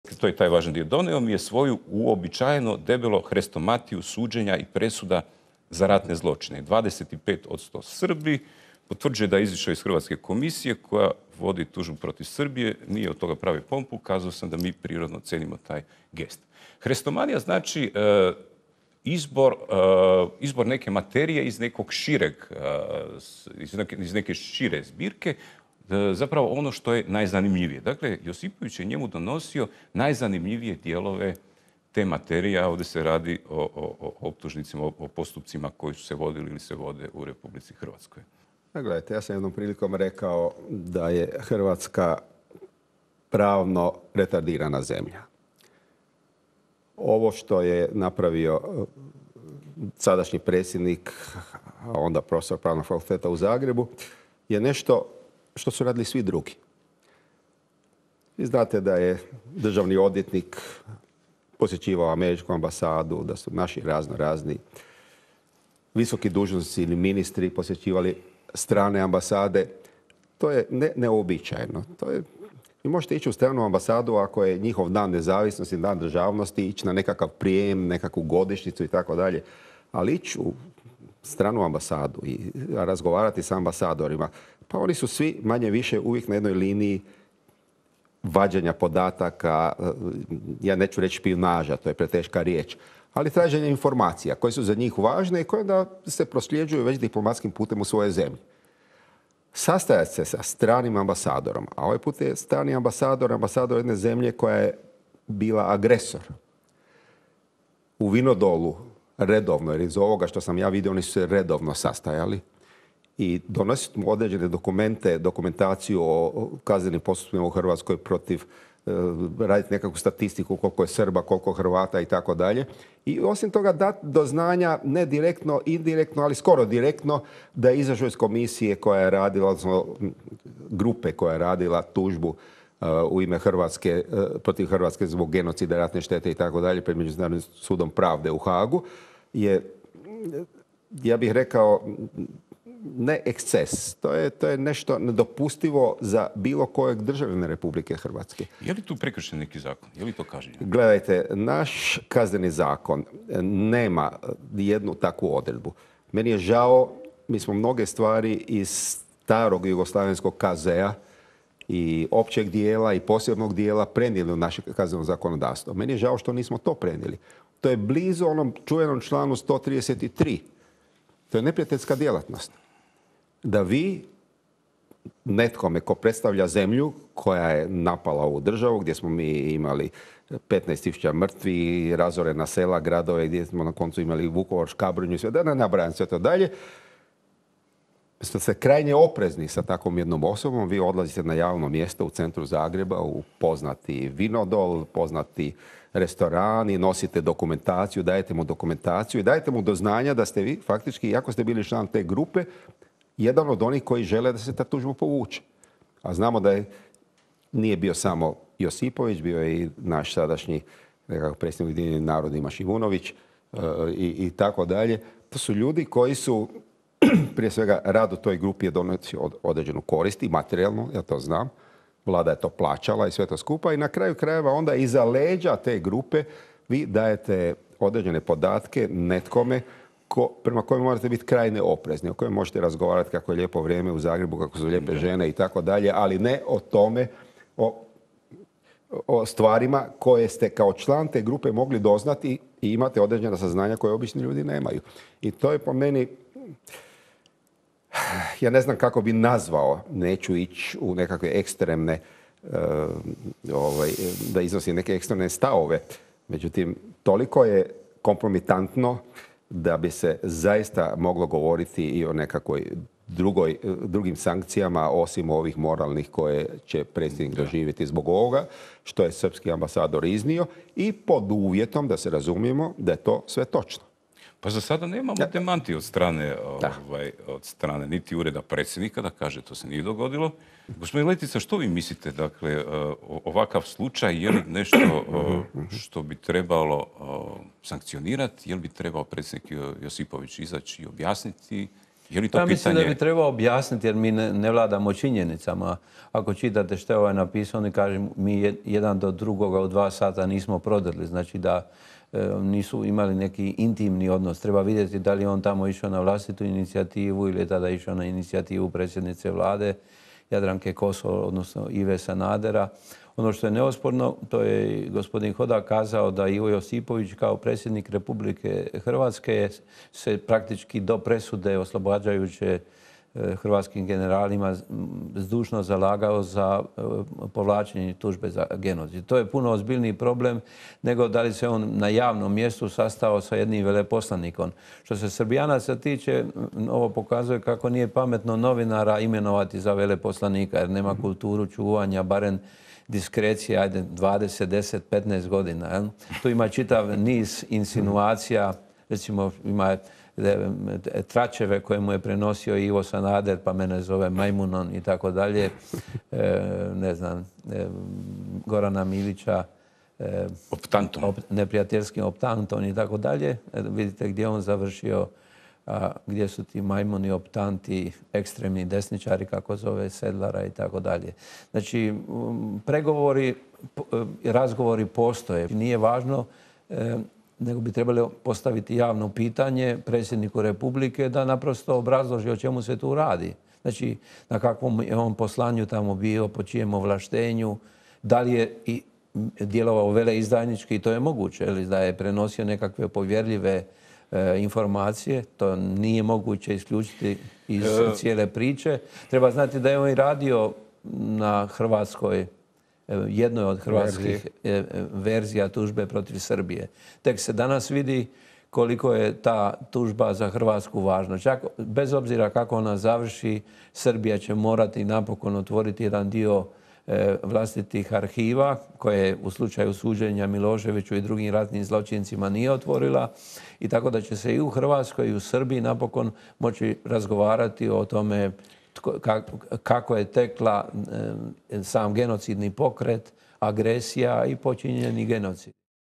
Je li Ivo Josipović bio doušnik srpskog veleposlanika R Cvetićanina?
Je li prof. Ivo Josipović, današnji predsjednik Republike Hrvatske bio doušnik Radovana Cvetićanina srpskog veleposlanika u Zagrebu od 2006. do 2009. godine? U knjizi Zagreb indoors R. Cvetićanin opisuje redovite susrete sa Ivom Josipovićem kojom prilikom mu Josipović, tadašnji član komisije za tužbu R. Hrvatske protiv R. Srbije pred međunarodnim sudom u Hagu, donosi "uobičajno debelu hretomatiju dokumenata"